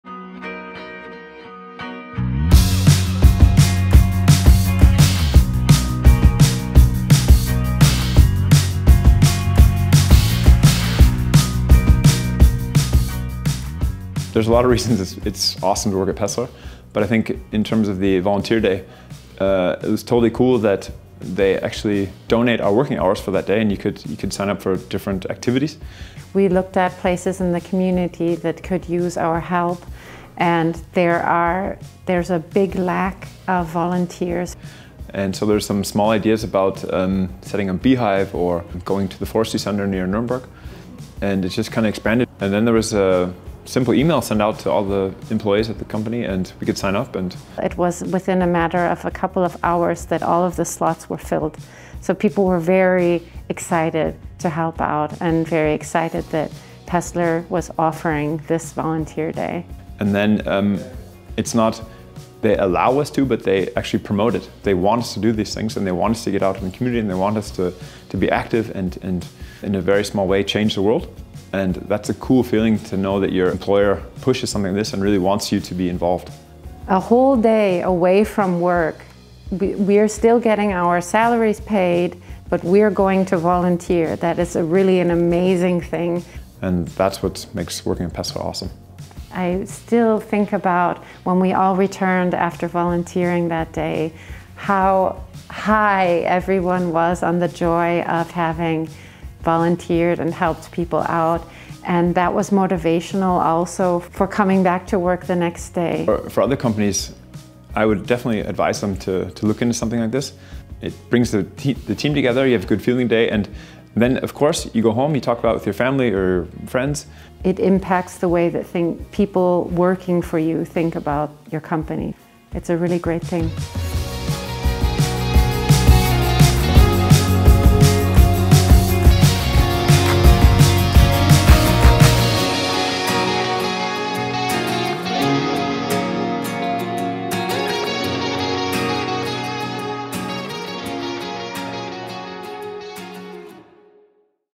Paessler Volunteer Day 2023
At Paessler, work isn't just about the nine-to-five grind. It's a place where employees find joy in what they do and the impact they make. This year, we celebrated our first-ever Volunteer Day, a day dedicated to giving back to the community. Join us in celebrating a day filled with compassion, teamwork, and the belief that even small actions can create significant change.

Watch the video and witness the power of Paessler's Volunteer Day 2023!

If you would like to join this awesome team and say hi!

A very special thank you goes out to our front and back office, visual communications, and our purpose team for organizing this incredible day and for their exceptional coordination. Their dedication and efforts made Volunteer Day a resounding success.